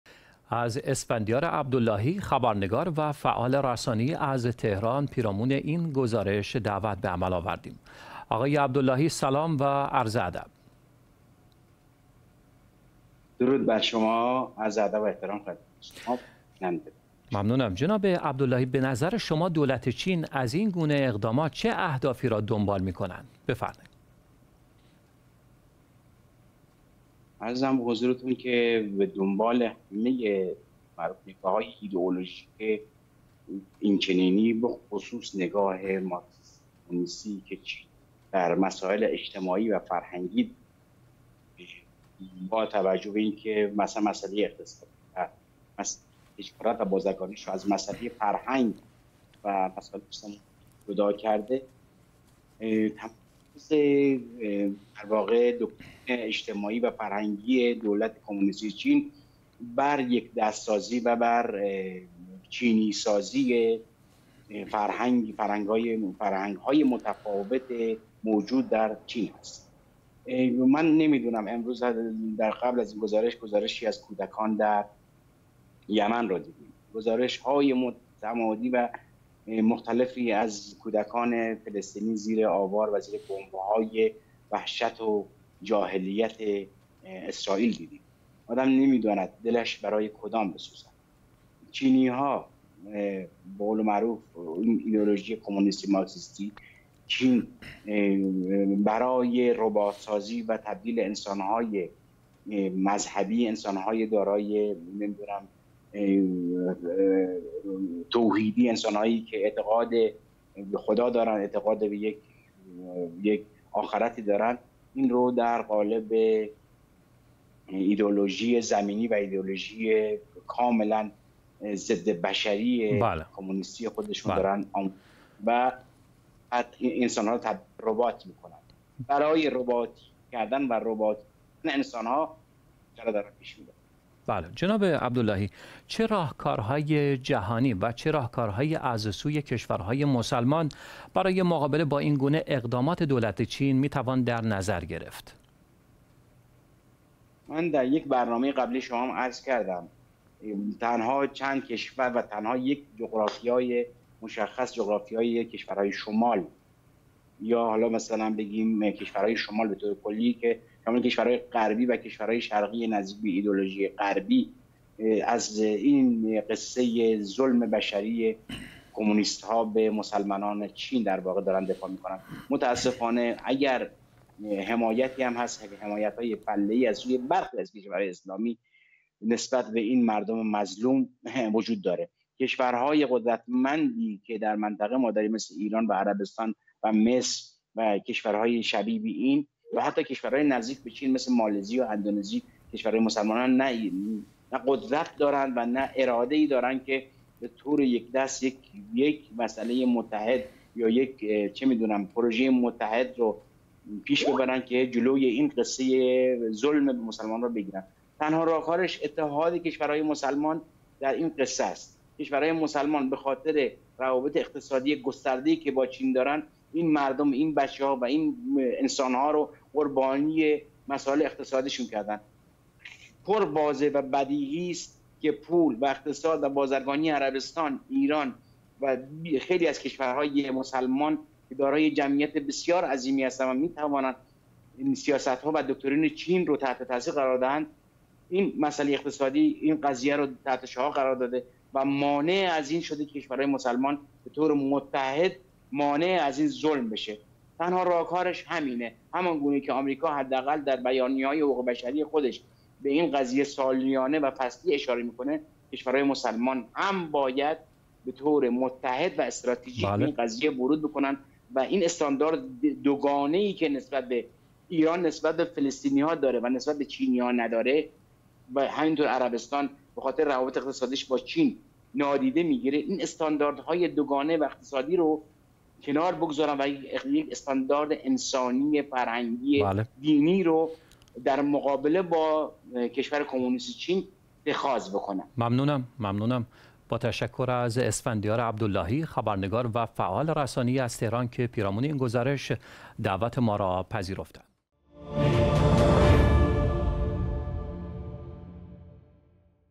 جنایات و سرکوب جدید چین علیه اویغورها - گفتگو با اسفندیار عبداللهی
جنایات و سرکوب چین علیه اویغورها به یکی از بزرگترین بحران‌های حقوق بشری جهان تبدیل شده است. گزارش‌های اخیر نشان می‌دهند که چین به طور سیستماتیک کودکان مسلمان اویغور را از خانواده‌هایشان جدا می‌کند تا آنها را از دین و فرهنگ خود دور کرده و به اجبار چینی‌سازی کند.
این گزارش‌ها توسط پژوهشگران مستقل و محققان دانشگاه کرنل تهیه شده و تاکید دارند که بزرگترین جنایت چین در حال حاضر، ربودن و جذب اجباری کودکان در منطقه مسلمان‌نشین ترکستان شرقی است. این اقدامات به عنوان بخشی از کارزار سرکوب گسترده‌تر چین علیه اقلیت‌های قومی و مذهبی، شامل قزاق‌ها و سایر قومیت‌های مسلمان، صورت می‌گیرد.

در گفتگوی اختصاصی با اسفندیار عبداللهی، خبرنگار و فعال رسانه‌ای از تهران، پیرامون جنایات و سرکوب چین علیه اویغورها بر اهداف و پیامدهای این سیاست‌های چینی‌سازی تاکید کرد. عبداللهی اشاره کرد که دولت چین با این اقدامات به دنبال محو هویت دینی و فرهنگی اویغورها و ایجاد یک نسل جدید از کودکان بدون ارتباط با ریشه‌های فرهنگی و مذهبی خود است.
وی همچنین به ضرورت واکنش جامعه بین‌المللی و کشورهای مسلمان برای مقابله با این جنایات تاکید کرد و پیشنهاد داد که از طریق فشارهای دیپلماتیک و تحریم‌های اقتصادی بر دولت چین، می‌توان این سرکوب‌ها را متوقف کرد. برای اطلاعات بیشتر و مشاهده گفتگوی کامل با اسفندیار عبداللهی، کلیک کنید و ویدیو را ببینید.

00:26 - پرسش از آقای عبداللهی درباره اهداف دولت چین
00:32 - پاسخ آقای عبداللهی درباره نگاه ایدئولوژیک و فرهنگی دولت چین
01:55 - پرسش درباره راهکارهای جهانی و کشورهای مسلمان برای مقابله با اقدامات دولت چین
02:23 - پاسخ آقای عبداللهی درباره راهکارهای جهانی و کشورهای مسلمان
04:11 - اشاره به استانداردهای دوگانه و نیاز به اتحاد کشورهای مسلمان
ویدیوهای مرتبط